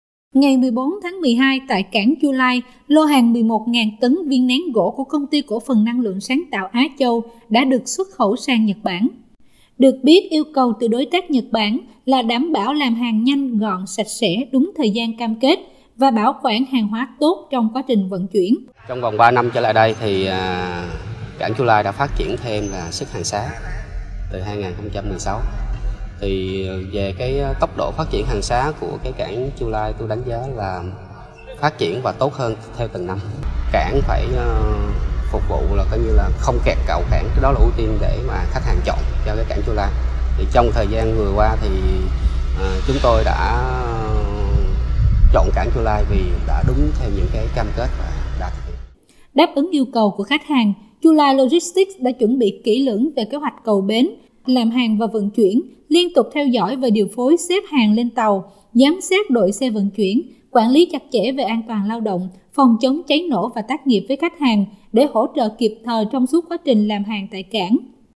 XUẤT KHẨU 100 CONTAINER HÀNG VIÊN NÉN TỪ CẢNG CHU LAI SANG NHẬT BẢN THÁNG 12/ NĂM 2019
Xuất khẩu 11.000 tấn viên nén gỗ từ cảng Chu Lai sang Nhật Bản
Ngày 14/12, tại cảng Chu Lai, lô hàng 11.000 tấn viên nén gỗ của Công ty Cổ phần Năng lượng sáng tạo Á Châu đã được xuất khẩu sang Nhật Bản. Thời gian tới, Công ty Công ty Cổ phần Năng lượng sáng tạo Á Châu sẽ đảm bảo khối lượng hàng hóa ổn định xuất qua cảng Chu Lai, từng bước trở thành đối tác chiến lược của Chu Lai Logistics trong việc xuất khẩu hàng viên nén tại khu vực miền Trung.
Ngày 14/12, tại cảng Chu Lai, lô hàng 11.000 tấn viên nén gỗ của Công ty Cổ phần Năng lượng sáng tạo Á Châu đã được xuất khẩu sang Nhật Bản.
Công ty Cổ phần Năng lượng sáng tạo Á Châu có trụ sở tại huyện Bình Sơn – tỉnh Quảng Ngãi, là doanh nghiệp chuyên sản xuất và xuất khẩu sản phẩm viên nén sang Hàn Quốc và Nhật Bản. Thời gian qua, Công ty đã nhiều lần xuất khẩu hàng viên nén qua cảng Chu Lai. Chất lượng dịch vụ vượt trội, chi phí hợp lý, đáp ứng yêu cầu khắt khe của đối tác là những lý do để Công ty lựa chọn cảng Chu Lai để xuất khẩu hàng hóa thay vì cảng Dung Quất (Quảng Ngãi) như trước đây.
Yêu cầu từ đối tác Nhật Bản là đảm bảo làm hàng nhanh, gọn, sạch sẽ, đúng thời gian cam kết và bảo quản hàng hóa tốt trong quá trình vận chuyển, đặc biệt là bố trí cầu cảng thông thoáng đảm bảo thời gian tàu cập bến. Đáp ứng yêu cầu của khách hàng, Chu Lai Logistics đã chuẩn bị kỹ lưỡng về kế hoạch cầu bến, kế hoạch làm hàng và vận chuyển, liên tục theo dõi và điều phối xếp hàng lên tàu, giám sát đội xe vận chuyển, quản lý chặt chẽ về an toàn lao động, phòng chống cháy nổ và tác nghiệp với khách hàng để hỗ trợ kịp thời trong suốt quá trình làm hàng tại cảng.
Ông Nguyễn Nhật Tiến – Giám đốc Công ty Cổ phần Năng lượng sáng tạo Á Châu đã đánh giá cao năng lực hoạt động và chất lượng dịch vụ của Chu Lai Logistics, đặc biệt là việc cung cấp các dịch vụ trọn gói, các tuyến vận chuyển hàng hải quốc tế trực tiếp từ Chu Lai đi Nhật Bản, Hàn Quốc và việc bốc dỡ, lưu trữ tại cảng luôn đảm bảo chất lượng, đúng tiến độ và thời gian giao hàng. Thời gian tới, Công ty Công ty Cổ phần Năng lượng sáng tạo Á Châu sẽ đảm bảo khối lượng hàng hóa ổn định xuất qua cảng Chu Lai, từng bước trở thành đối tác chiến lược của Chu Lai Logistics trong việc xuất khẩu hàng viên nén tại khu vực miền Trung.